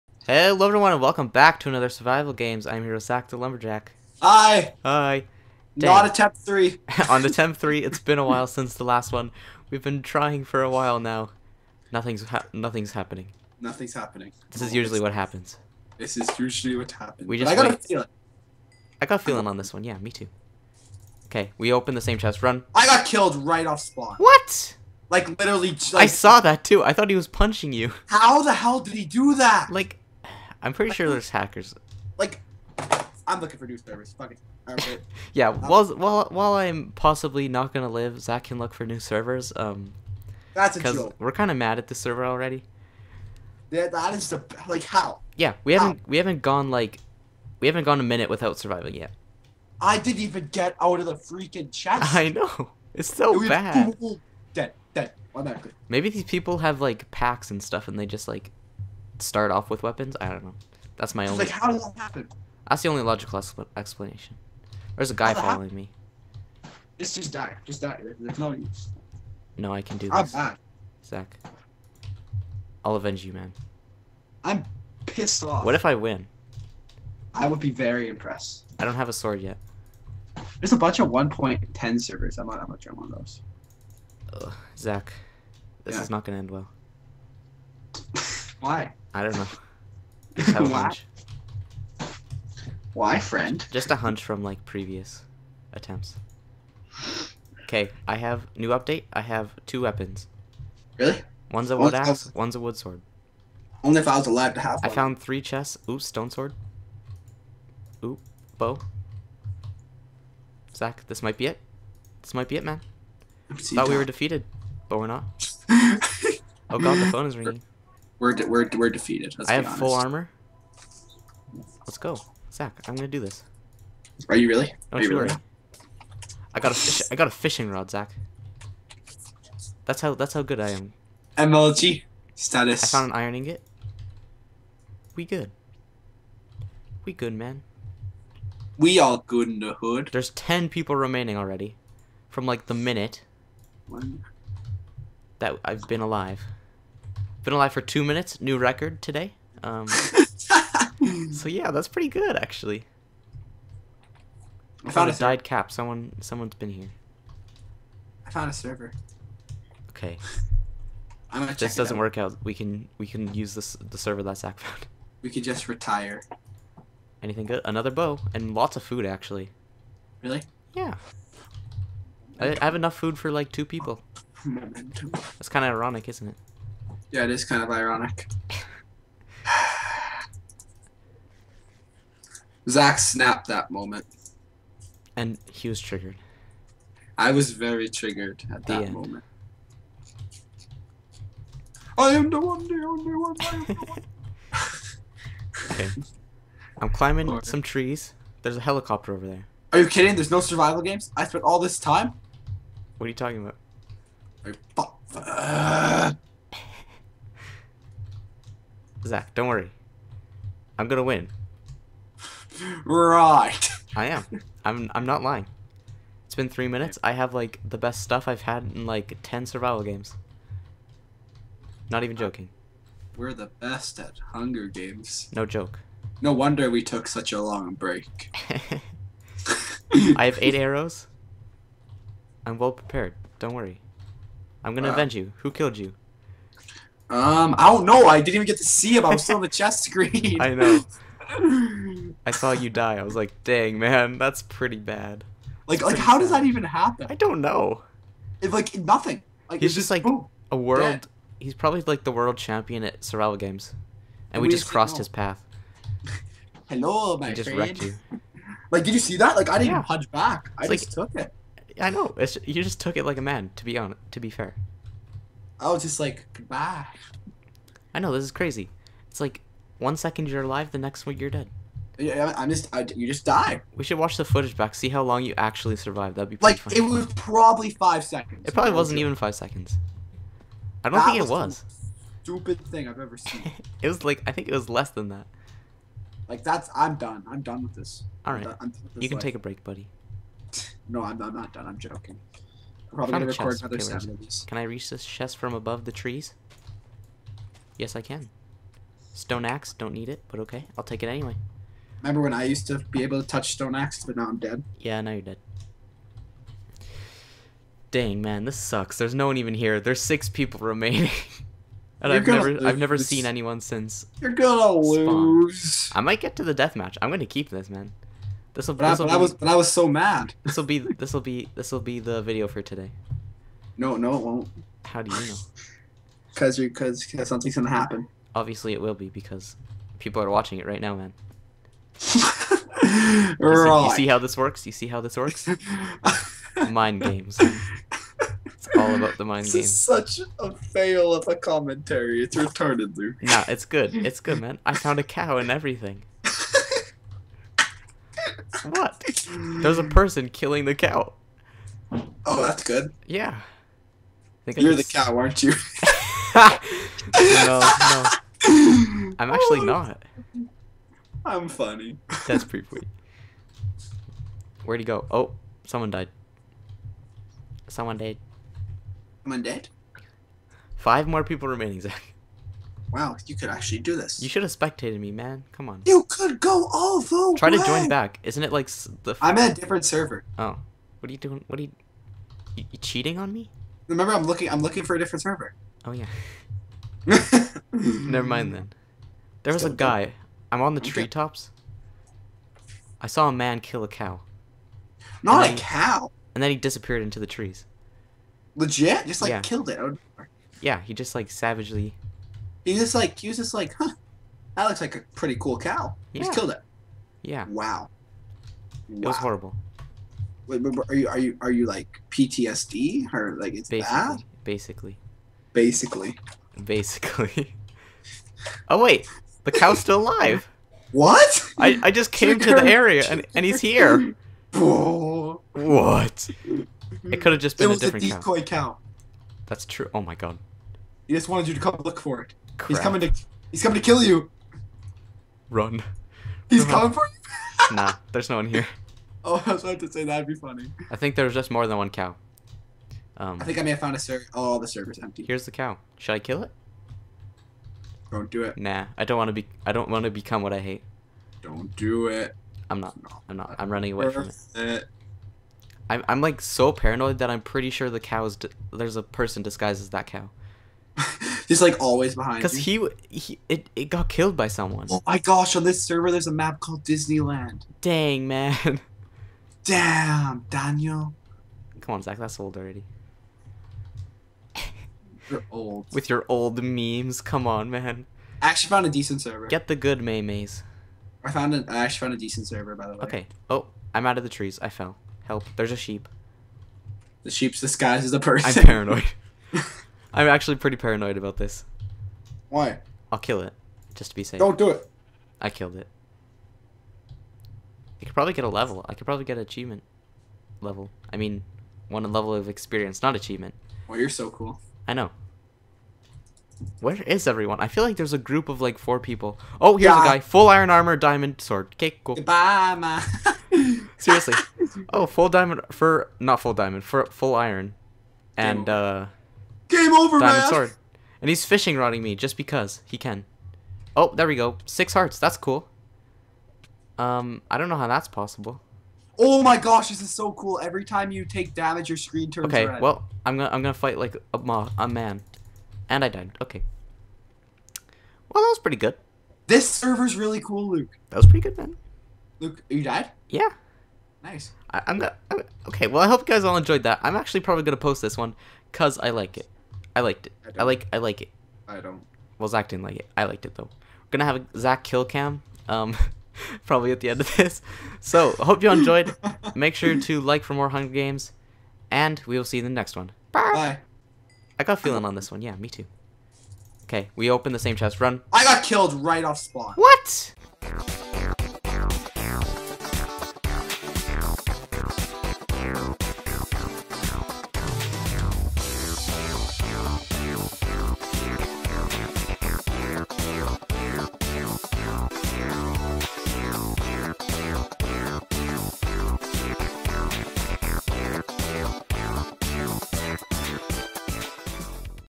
Survival Games Ep 34 - THE FASTEST DEATH!!!!
Today Zach and I play another survival games and one of us doesn't last very long (it doesn't take that long to find out).